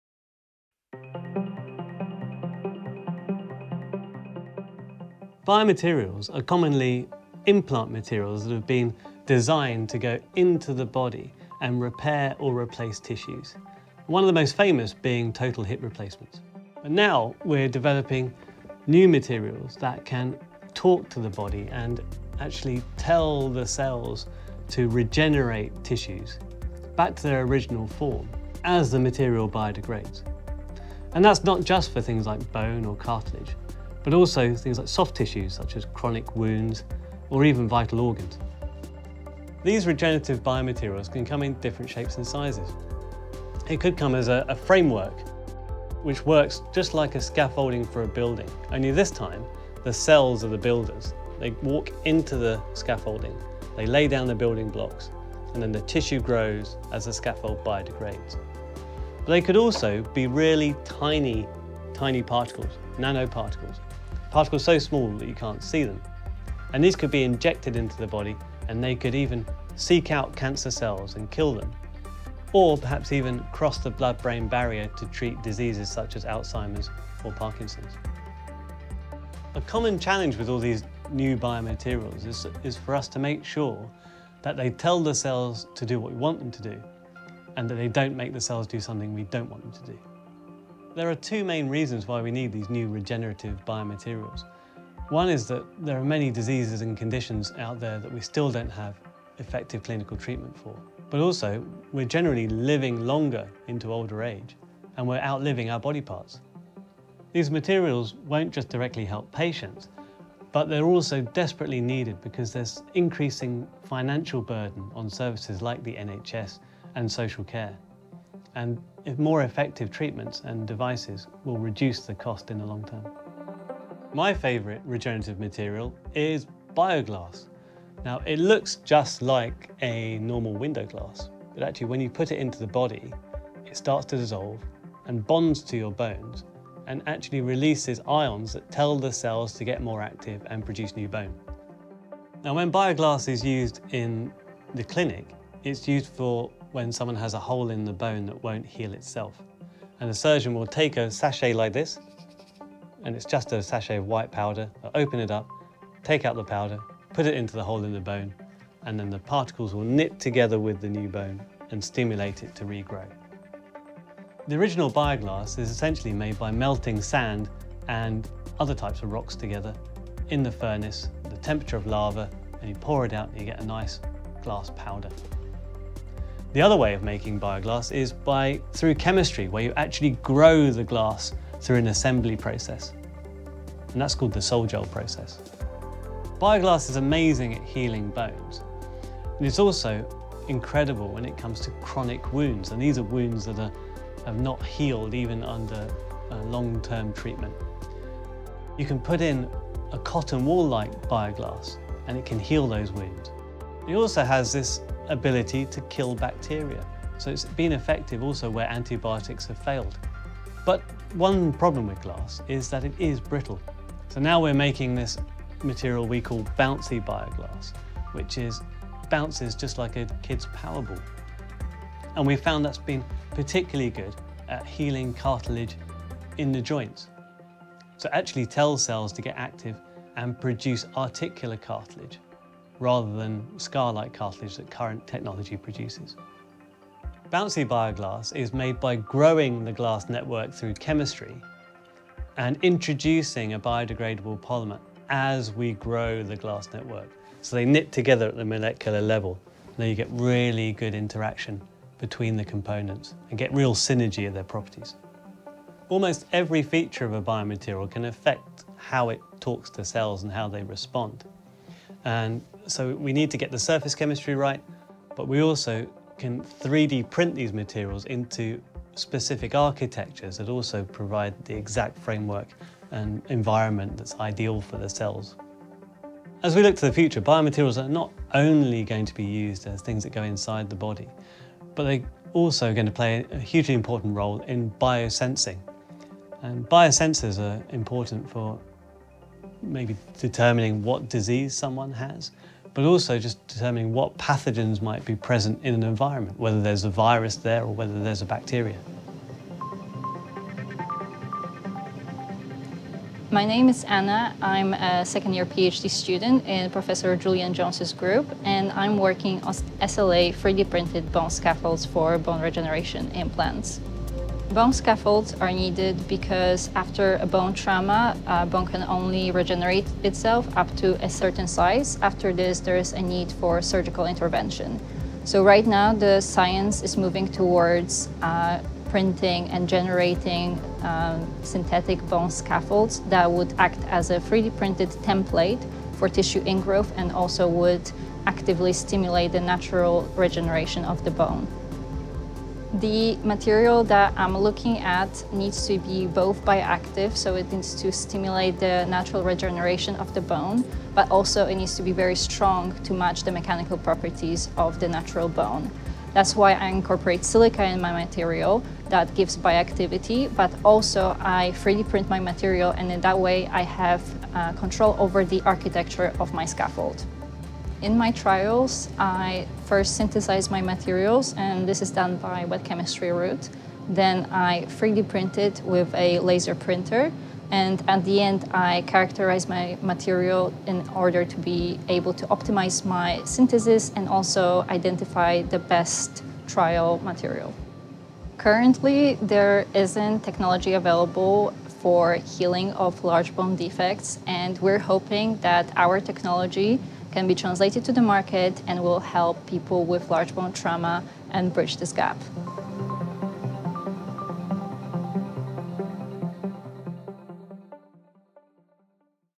What are biomaterials and how can they influence the future of healthcare?
It's NationalEngineeringDay!

Every day we work on projects to EngineerBetterLives, from new materials for healthcare to clean energy and safer environments.

Today we launch the first video in a new research series, starting with Professor Julian Jones and Anna Tchorzewska. In this video, they explain more about their research into biomaterials for healing cartilage in the joints and  bone regeneration.